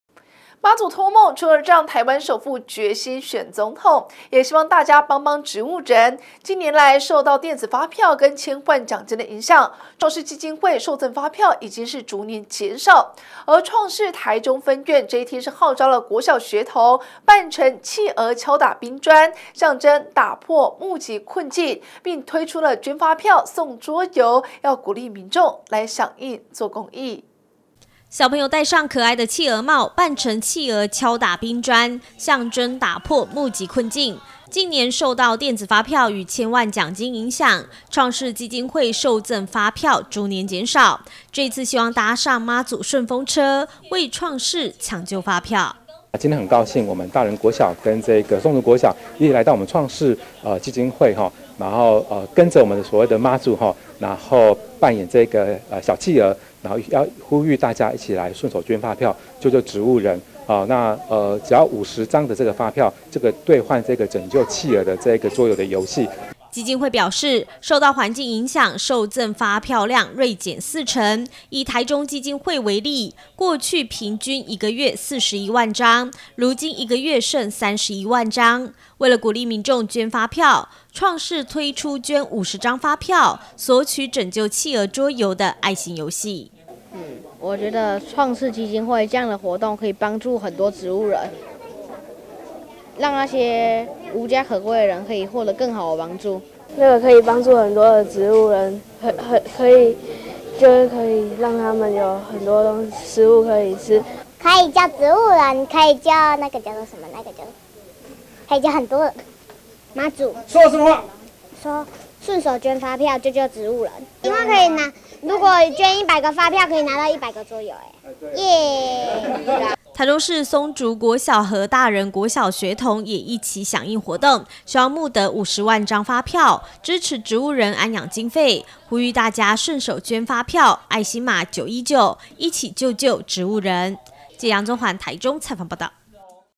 大台中新聞-捐發票贈送桌遊 幫助植物人安養
媽祖託夢，除了讓台灣首富決心選總統，也希望大家幫幫植物人，近年受電子發票與千萬獎金影響，創世基金會受贈發票逐年減少，創世台中分院今天號召國小學童扮成企鵝敲打冰磚，象徵打破募集困境，並推出捐發票送桌遊，鼓勵民眾響應做公益 。 「以上新聞可由top NEWS APP收看」